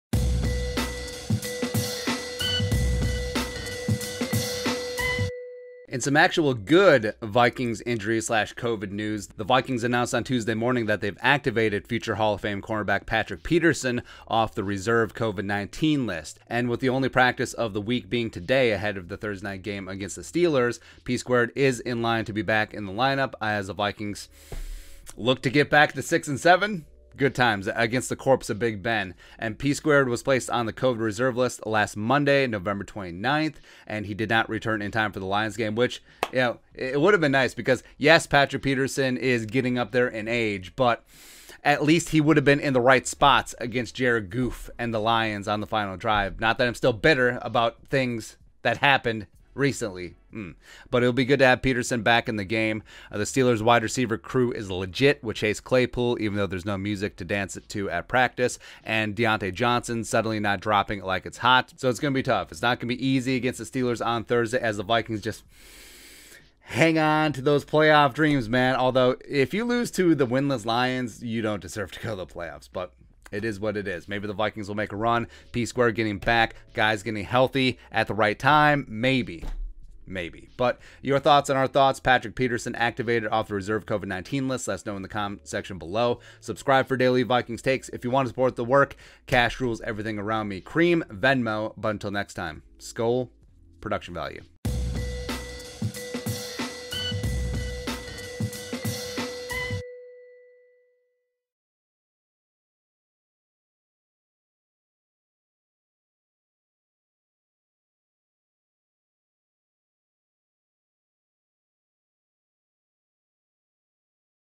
Vikings Activate Patrick Peterson Off Reserve/COVID-19
The Minnesota Vikings have activated future Hall of Fame cornerback Patrick Peterson off Reserve/COVID-19 and P2 is in line to play Thursday versus the Pittsburgh Steelers.